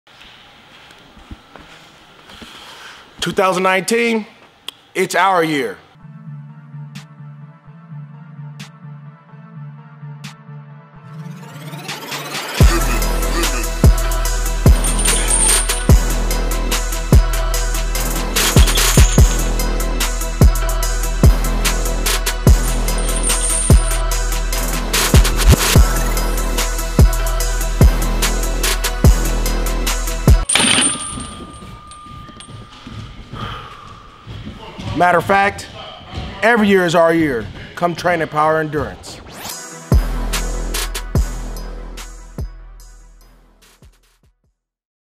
2019 is Our Year - Power Endurance Training Center
Want to know what Power Endurance Training Center has prepared for the new year? Watch the video to see how we at PETC are preparing for this year's challenges.